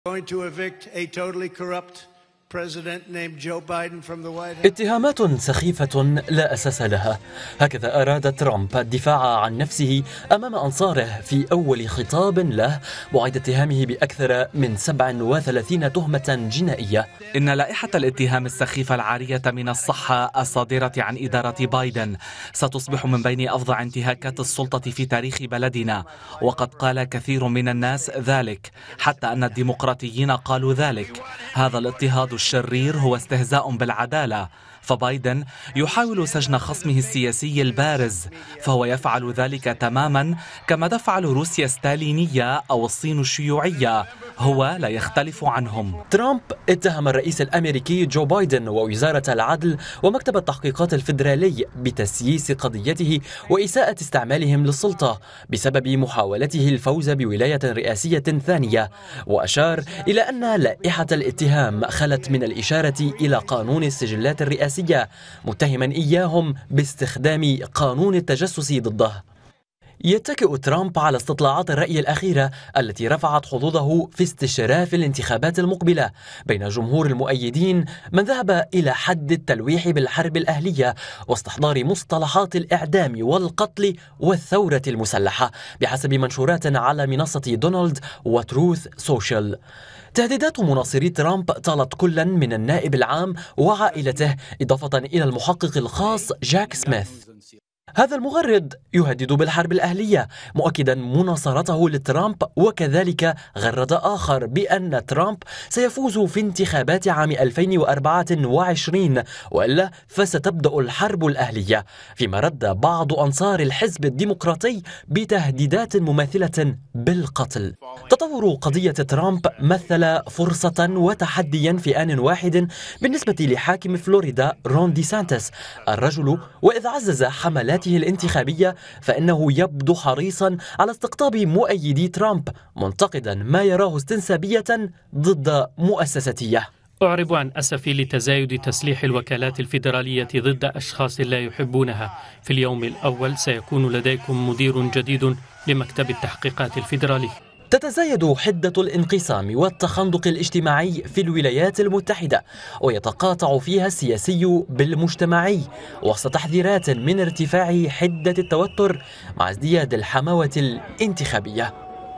ترامب يتهم بايدن ووزارة العدل بتسييس قضيته
الرئيس الأميركي السابق دونالد ترامب يتوجه إلى أنصاره للمرة الأولى بعد توجيه لائحة اتهامات له متهمّا جو بايدن ووزارة العدل بتسييس قضيته فيما تعلو أصوات داخلية مهددة بالحرب الأهلية

إعداد: مهدي ديراني